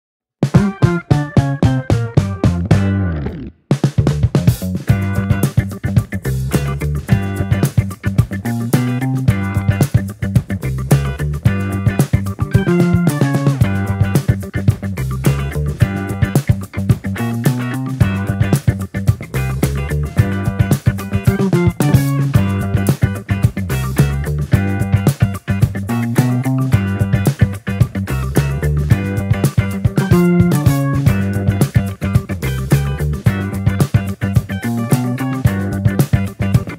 CuttanaNir2 for After Effects Quick Start
Toolkit for creating write-on animations. Featuring 5 exciting new features.